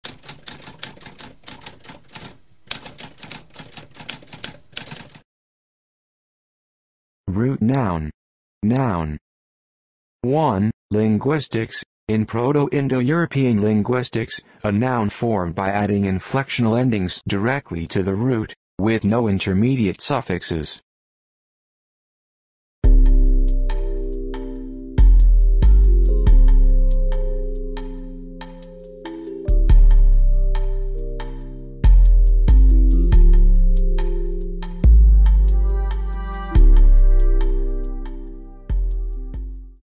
What does root noun mean?
A spoken definition of root noun.

Intro Sound:
Typewriter - Tamskp